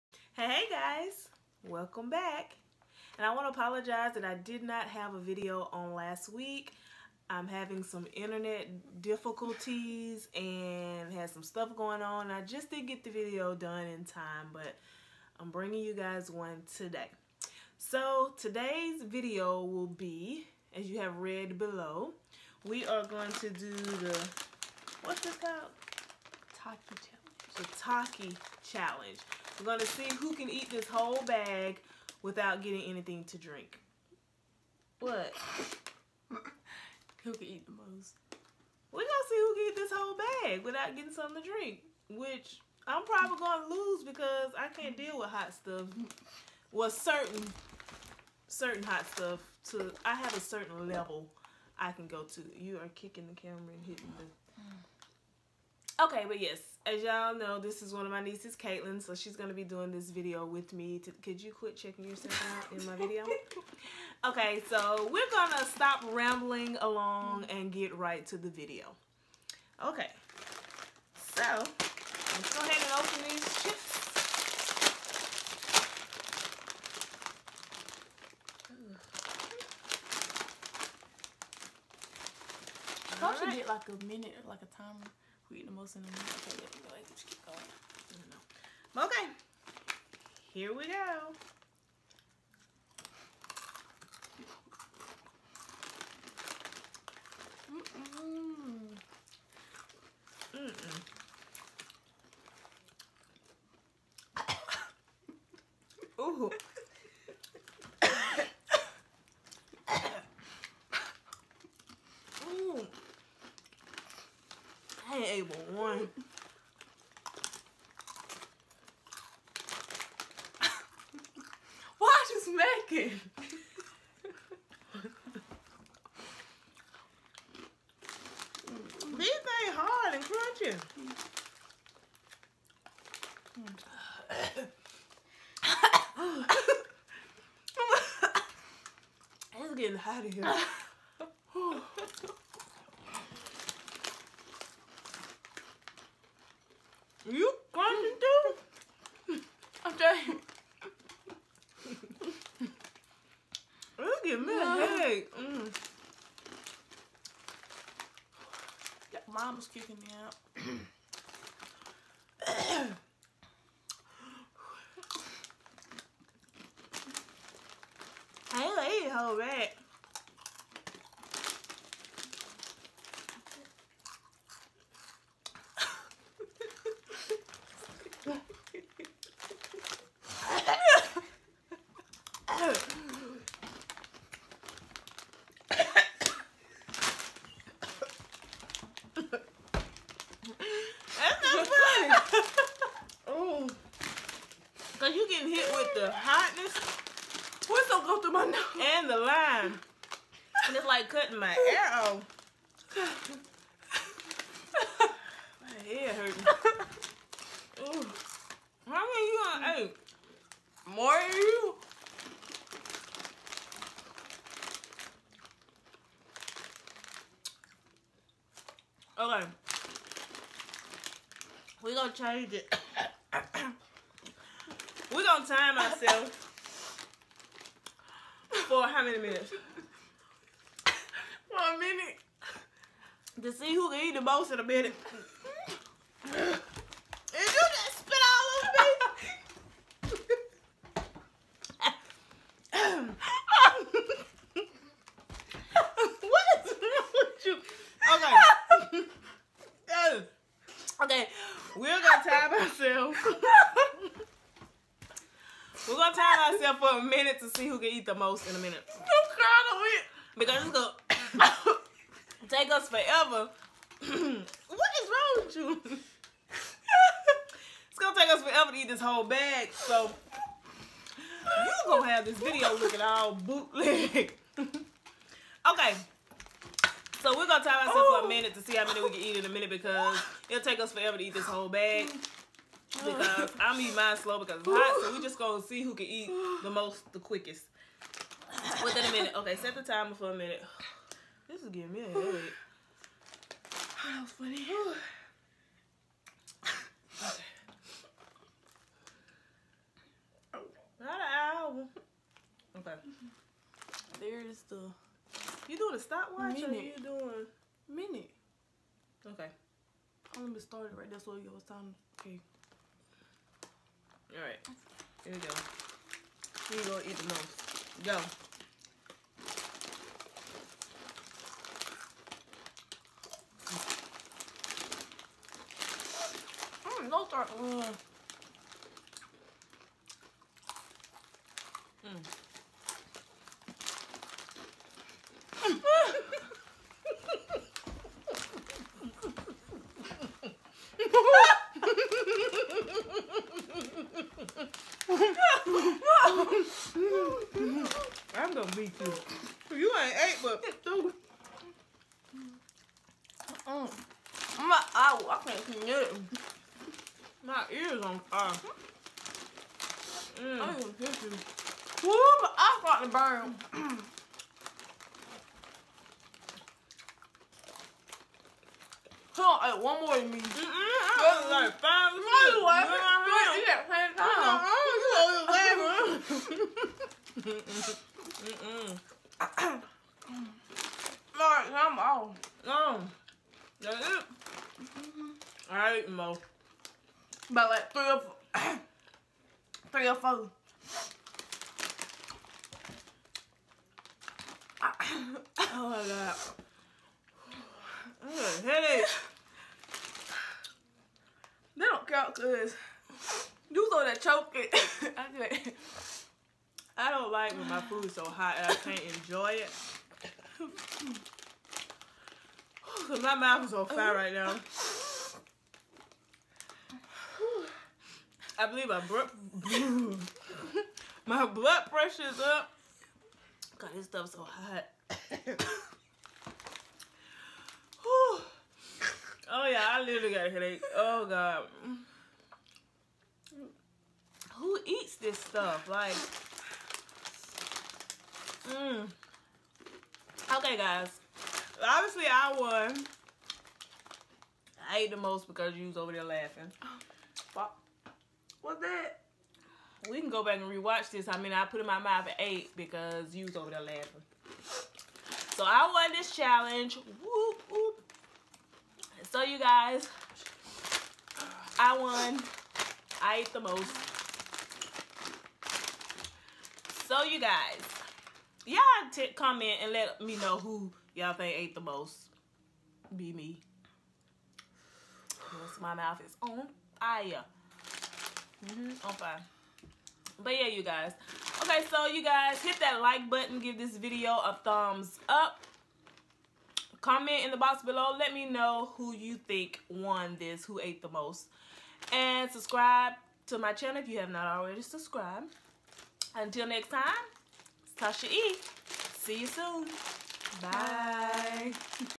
Takis Challenge Who Can Eat The Most
Me and my niece Katlyn doing the Takis Challenge!!